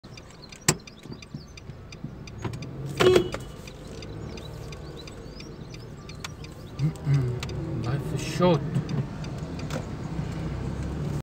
The light for the pedestrians was red when the robot turned green for me.
The light was red for the pedestrians when the robot turns green for me to proceed

as I was about to proceed the pedestrians wanted to take a chance and run across the road

so I found out the car's horn early to avoid an accident

but one of the pedestrians made a very dumb move